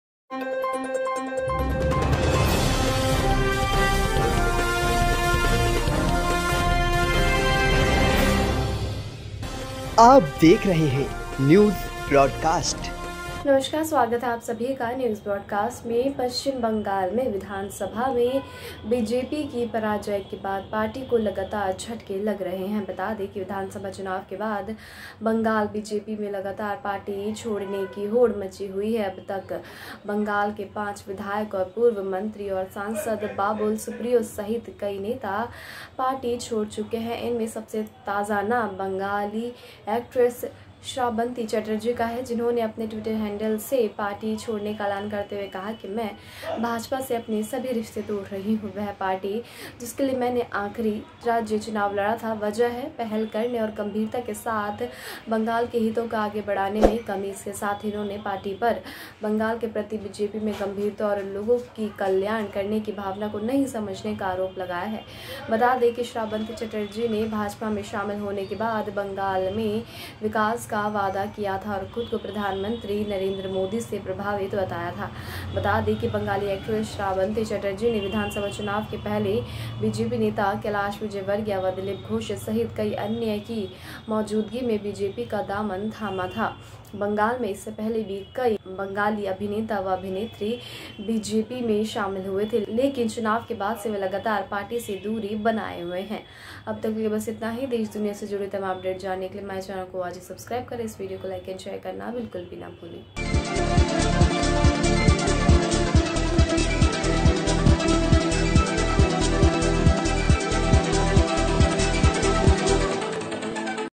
BREAKING! Big Blow To BJP In West Bengal | Actress Srabanti Chatterjee Resigns
पश्चिम बंगाल में विधानसभा में बीजेपी की पराजय के बाद पार्टी को लगातार झटके लग रहे हैं. बता दें कि विधानसभा चुनाव के बाद बंगाल बीजेपी में लगातार पार्टी छोड़ने की होड़ मची हुई है. अभी तक बंगाल के पांच विधायक और पूर्व मंत्री और सांसद बाबुल सुप्रियो सहित कई नेता पार्टी छोड़ चुके हैं। इनमें सबसे ताजा नाम बंगाली अभिनेत्री श्राबंती चटर्जी का है जिन्होंने अपने ट्विटर हैंडल से पार्टी छोड़ने का ऐलान करते हुए कहा, "मैं भाजपा से अपने सभी रिश्ते तोड़ रही हूं।